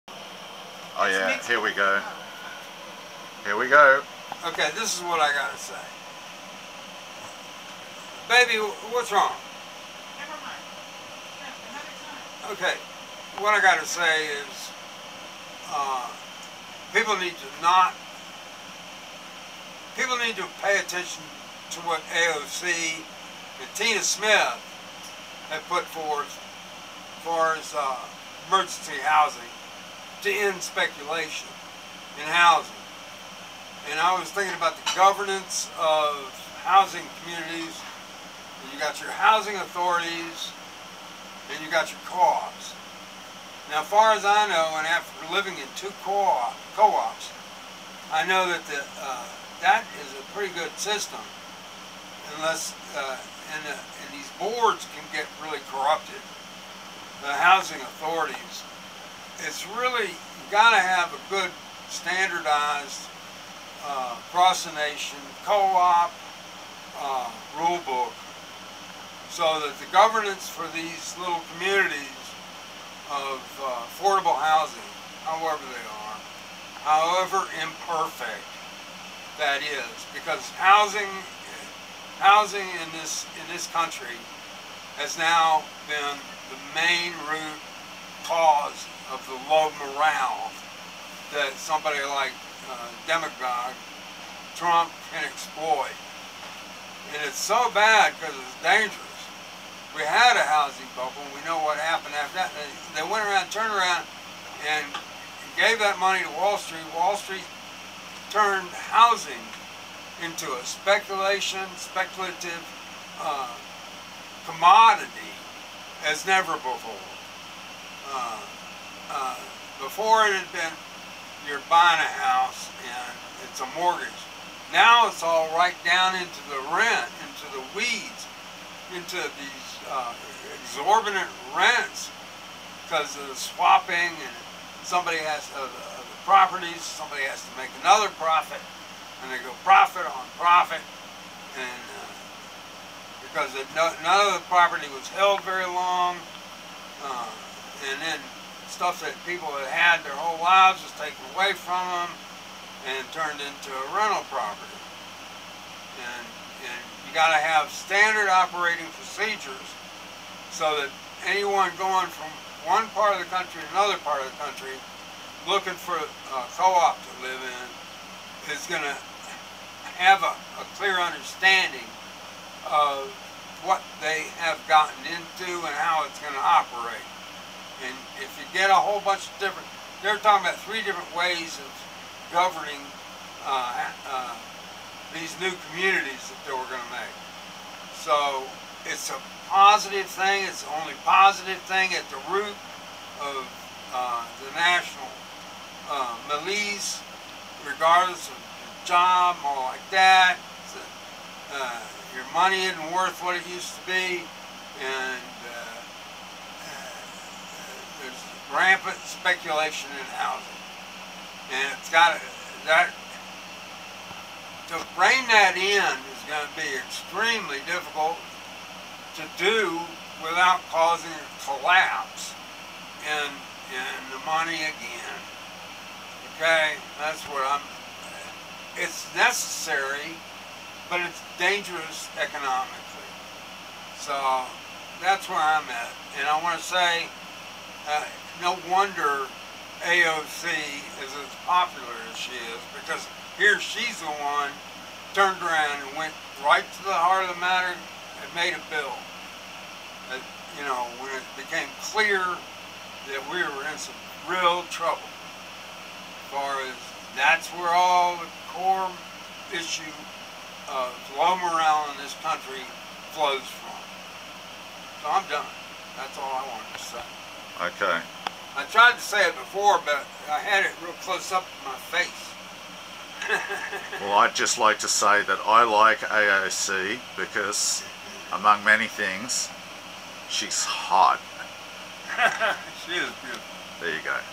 AOC Housing Bill Housing is the Source of LOW, Morale /Transcendian
The bit I have done with my mate Bruce McClaren on camera may need to be turned up. I forgot to turn on my microphone.  Episodes forming a larger documentary with asides of simple short art films. Documentary of a Small Nation was the VHS tape .